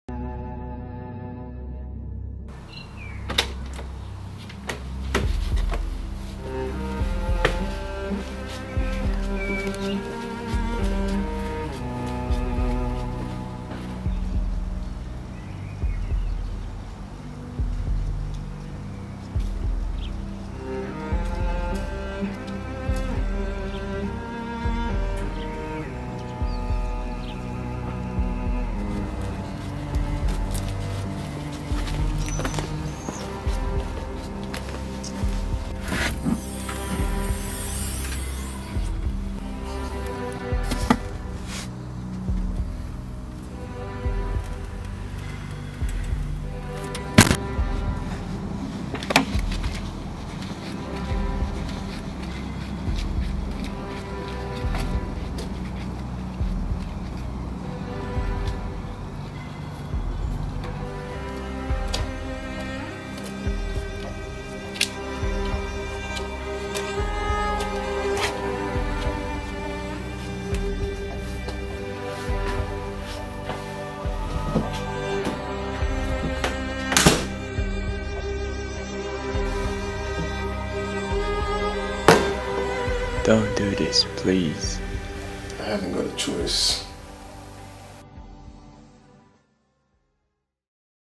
Short Feature part 2
This is the second scene of the short feature. As it is still an experiment, these test shoots help us a lot to develop our story more and more and change some of the shots, which we think do not work. Also with the feedback we get we are trying to change and improve our shots in the next scenes.